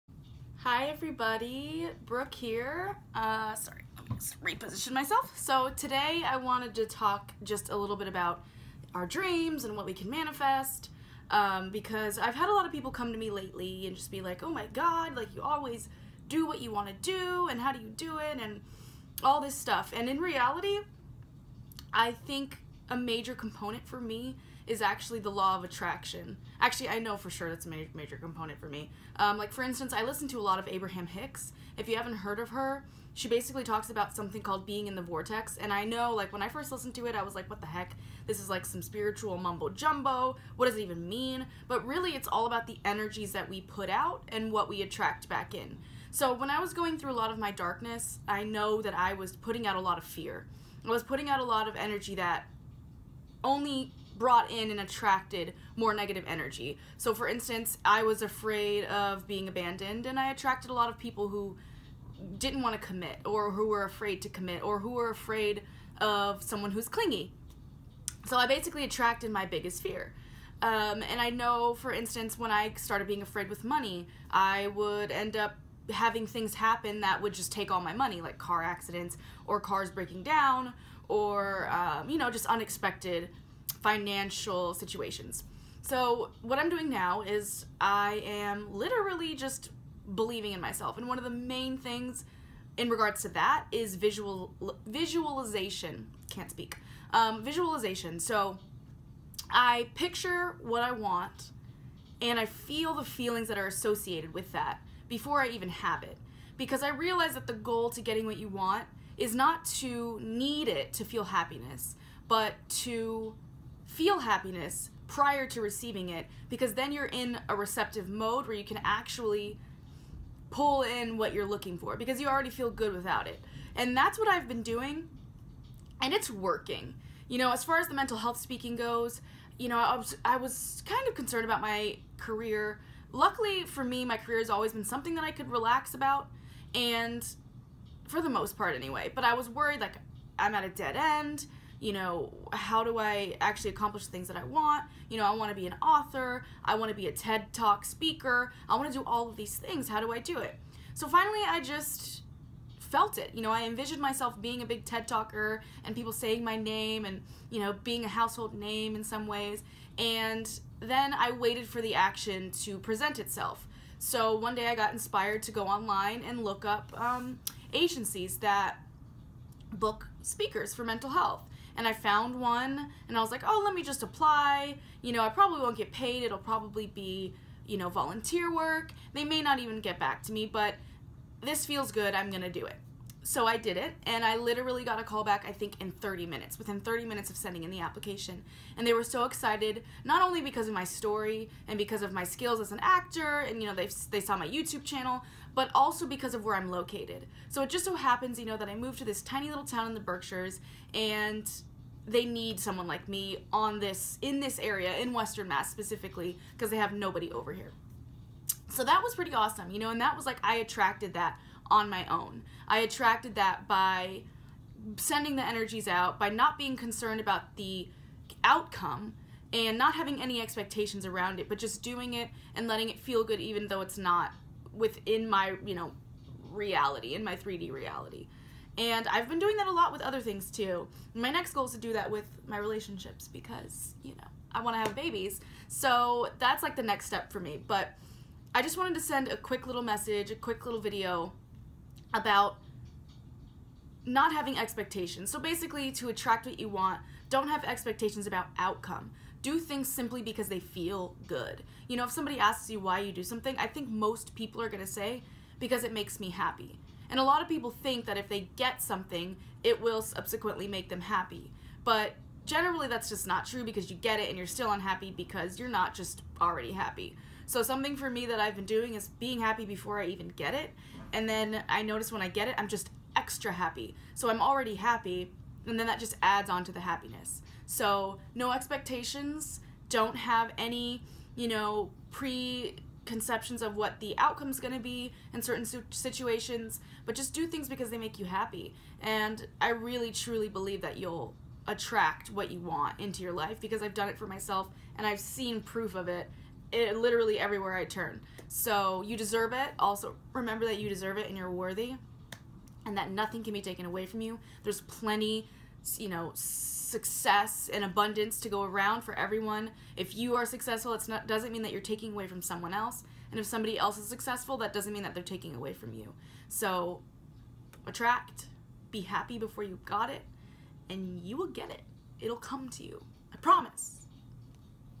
How I Use Law of Attraction | Brooke Bridges
I listen to a lot of Abraham Hicks, and I utilize her concept and teachings of Law of Attraction. I've always attracted what I've put out, and everyone else does too! If I'm feeling fear, I'll attract things that cause me to be fearful. If I feel security and clarity, I'll attract things and people into my life that inspire those feelings.

Give visualization a shot - check out my video next week for a more in-depth look into my visualization practice via journaling.